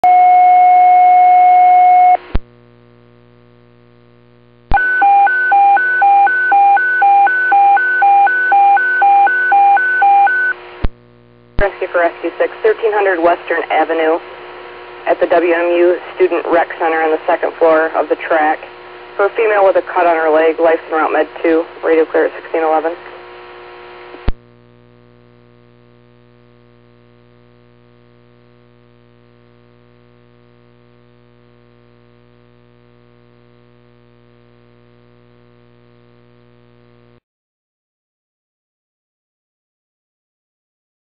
Station 6 Dispatch Kalamazoo Mon, 21 Nov 2016 16:11:25 -0500
Kalamazoo Department of Public Safety Dispatch for Station 6.  Tone recieved at Mon, 21 Nov 2016 16:11:25 -0500